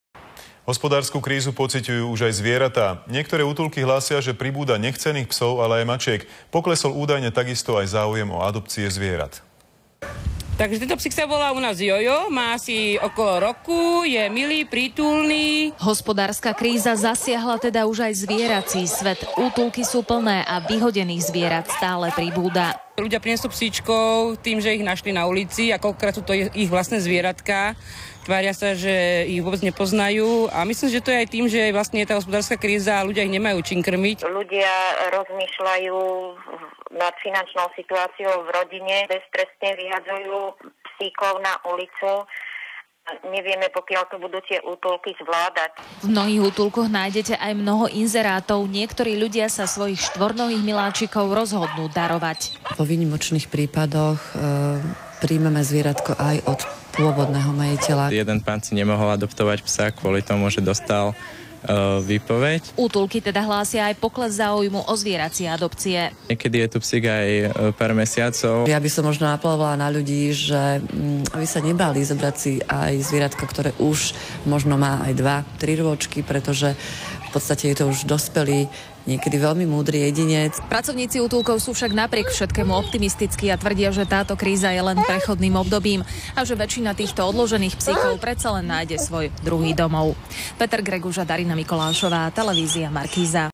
Kríza zasiahla už aj útulky
Reportáž TV Markíza o vplyve krízy na útulky pre zvieratá